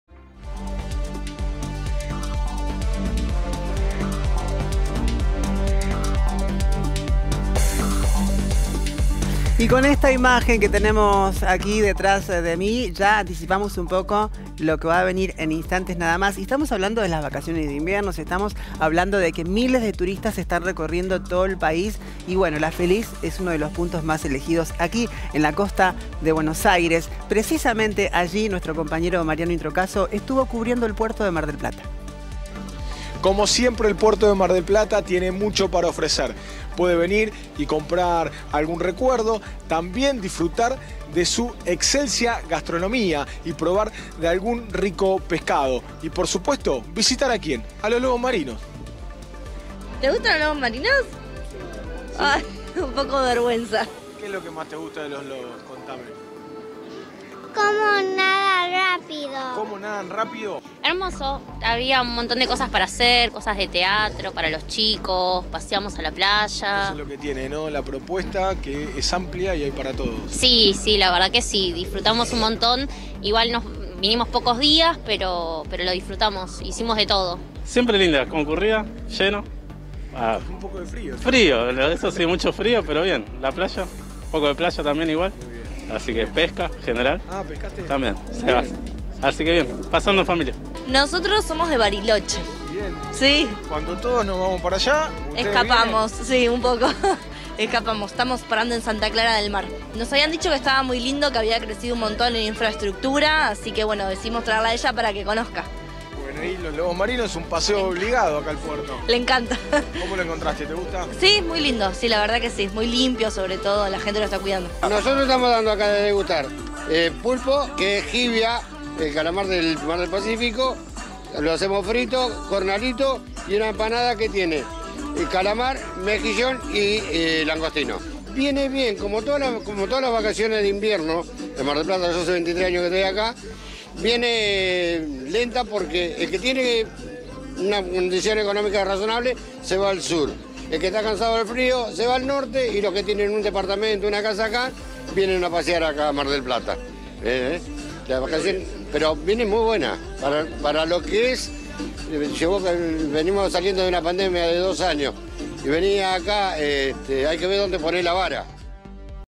Miles de turistas disfrutan de Mar del Plata
La ciudad balnearia está a pleno en vacaciones de invierno. Visitamos el puerto y probamos los sabores típicos.